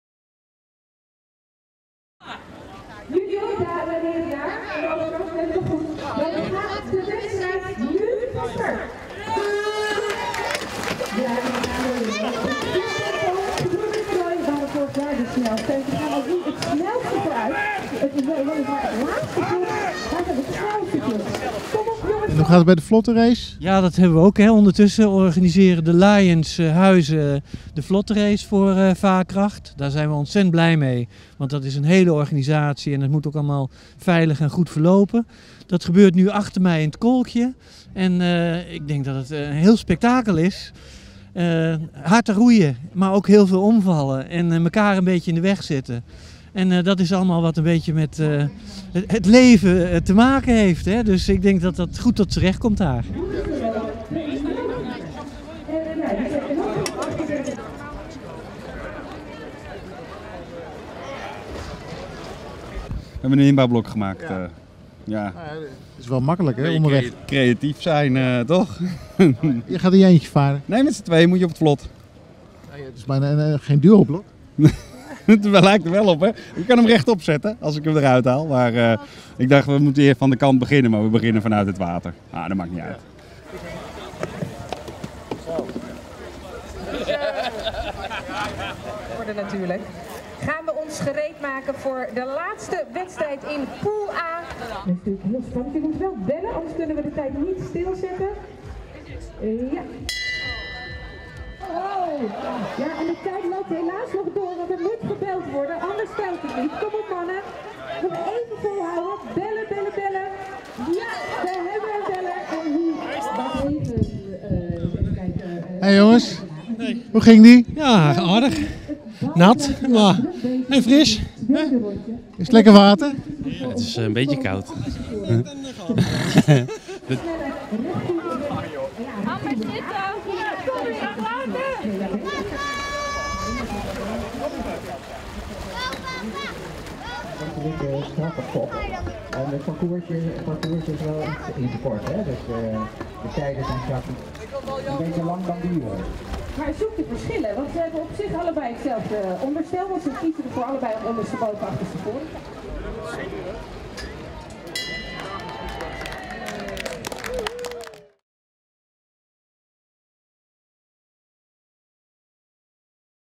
Vaarkracht vlottenrace 2015
Zaterdag 19 september werd de Vaarkracht vlottenrace gehouden georganiseerd door de Lions Huizen @lionsclubnederland
met een opbrengst van € 2500 bestemd voor het varen met kankerpatiënten en naasten.